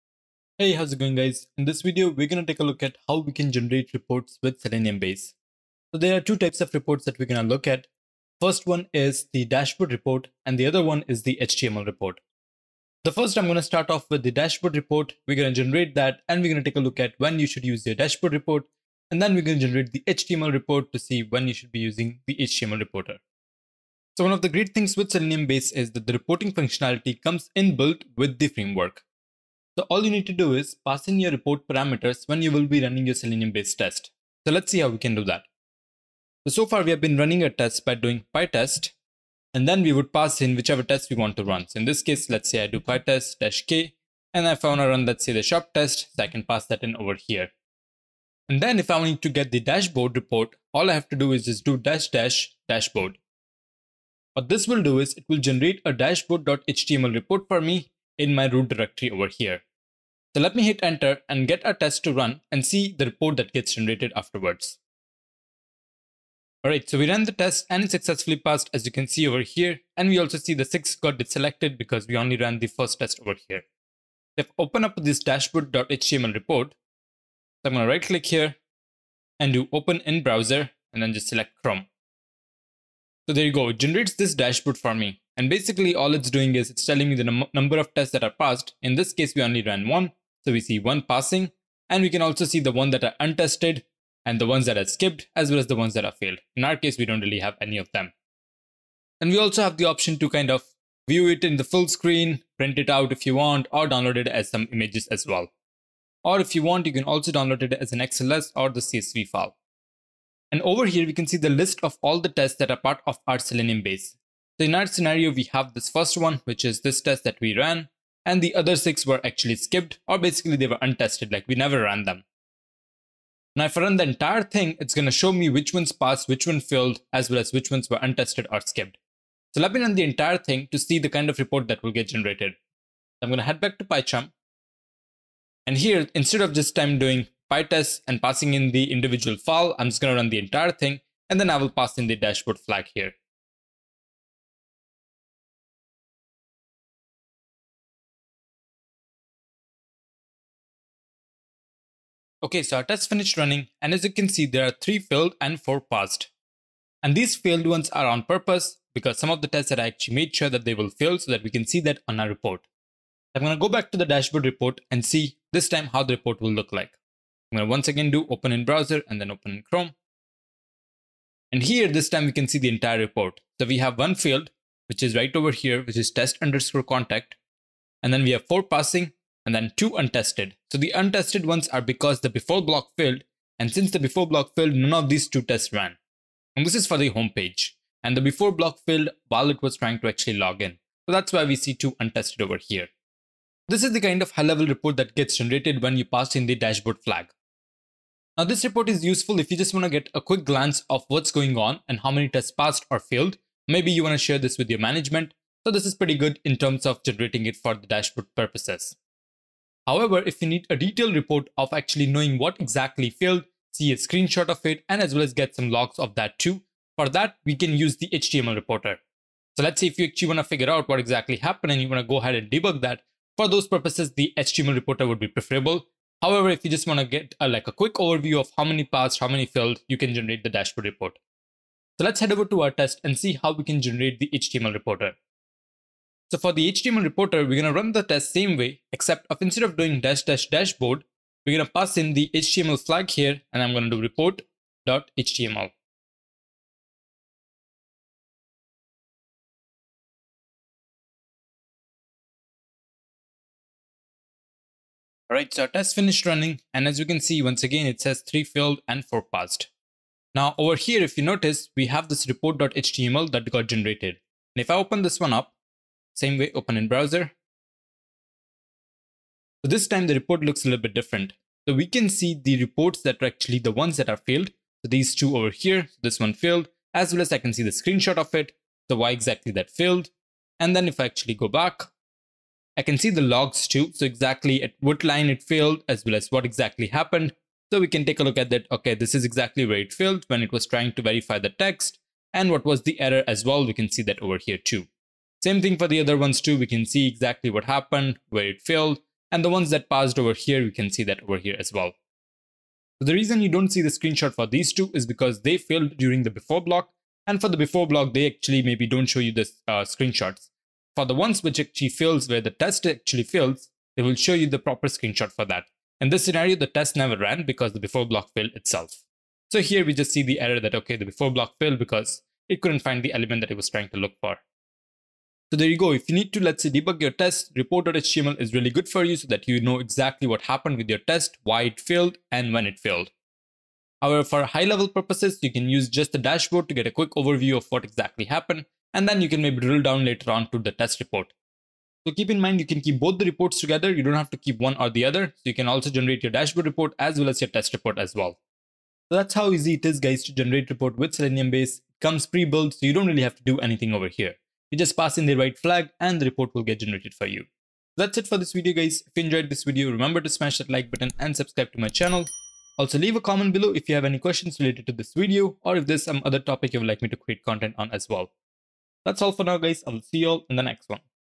Generate reports with SeleniumBase | Selenium Python Tutorial
In this Selenium Python tutorial, we will generate dashboard and HTML reports with the SeleniumBase framework.

Chapters:
0:00 - Generate reports with SeleniumBase
0:21 - Generate Dashboard Report
3:49 - Generate HTML Report
5:23 - Review

Automation Bro